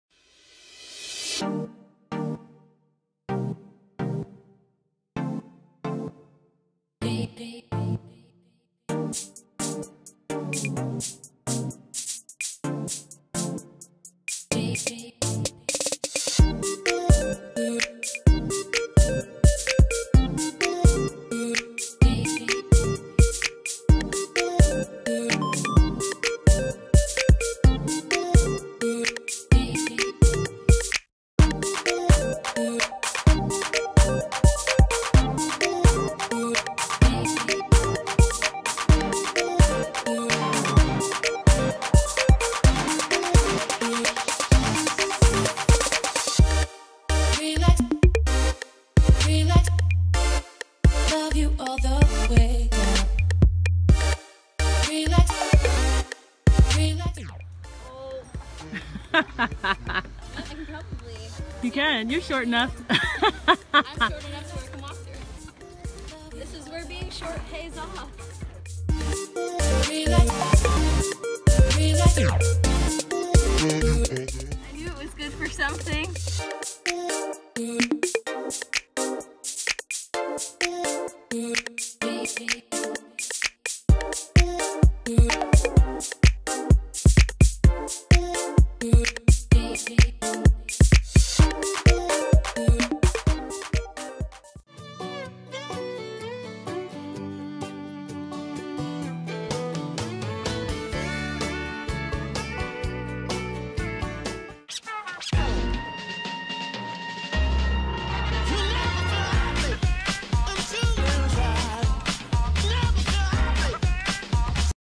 Come with me to... Go Ape! Treetop Adventure
For Father's Day, my parents and I decided to go to Go Ape! Go Ape is a treetop adventure course with six stages of platforms, climbing ladders, rope swings, ziplines, and a bunch of other stuff. I loved it all...

...except for the cargo nets... the cargo nets can go to hell...

Wishing a happy Father's Day to all the fathers out there, including my own "faja"! Love you, Daddy!